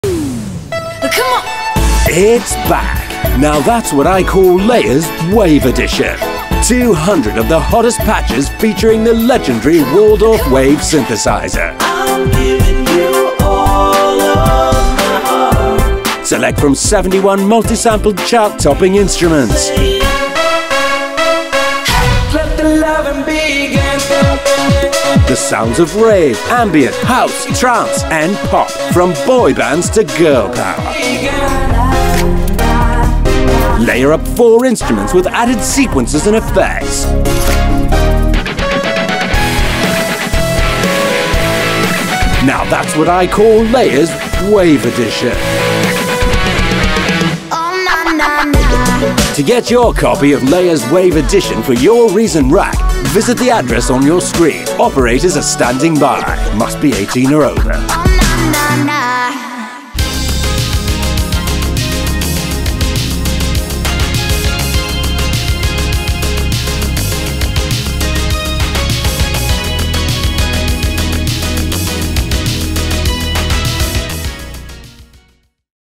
Introducing Layers Wave Edition
Layers Wave Edition brings you the sound of the mighty Waldorf Wave synthesizer in a modern, easy to use and abuse package. Four layers of unique samples from the Waldorf Wave combined with effects, unique sequencing capabilities and comprehensive modulation adds up to a powerful performance instrument, ready to evoke the futuristic sounds from a bygone era.Layers Wave Edition comes packed with hundreds of inspiration­inducing presets crafted by leading sound designers. The straightforward user interface and top­notch sample­set also invites you to experiment on your own and create sounds from scratch. Synthetic, evolving, dreamy, soft or rock hard – Layers Wave Edition has you covered.